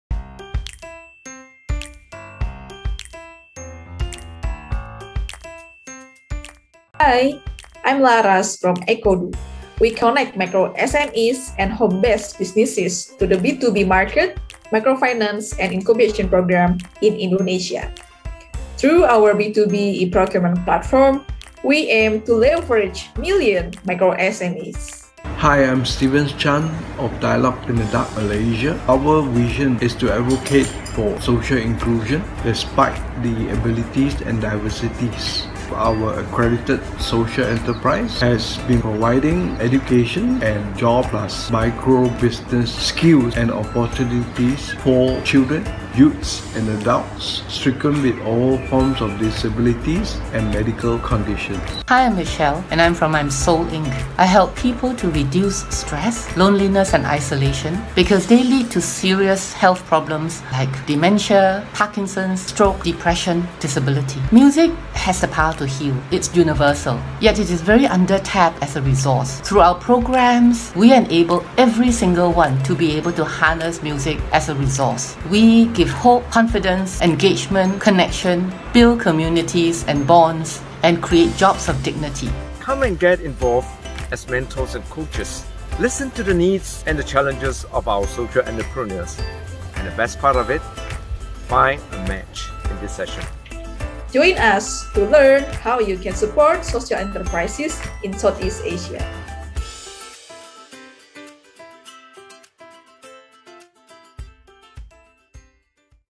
Meet the Changemakers on 12 March 2021, 5 - 6.30pm SGT
Meet the Changemakers (hosted by TBNAsia ExpertiseNetwork) on Fri 12 March 202, 5 - 6.30pm SGT.

A social entrepreneur's path can be lonesome without the support of trusted mentors, coaches and advisors to help them overcome the challenges ahead. Meet Michelle from I’mSoulInc, Laras from Ecodoe and Stevens from DialogueintheDark - the Changemakers from our SETH Cohort - and find out how you can play a vital role in their journey.

Session Objectives
1. learn about their work and impact communities
2. understand their needs
3. find your role to support these Changemakers